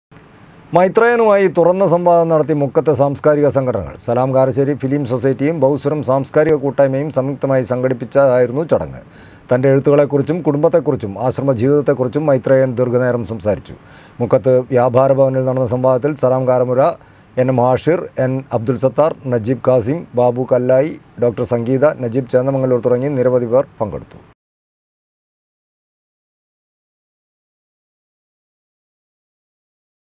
മൈത്രേയനുമായി തുറന്ന സംവാദം നടത്തി മുക്കത്തെ സാംസ്കാരിക സംഘടനകൾ
മൈത്രേയനുമായി തുറന്ന സംവാദം നടത്തി മുക്കത്തെ സാംസ്കാരിക സംഘടനകൾ. സലാം കരശ്ശേരി ഫിലിം സൊസൈറ്റിയും ബഹുസ്വരം സാംസ്‌കാരിക കൂട്ടായ്മയും സംയുക്തമായി സംഘടിപ്പിച്ചതായിരുന്നു ചടങ്ങ്.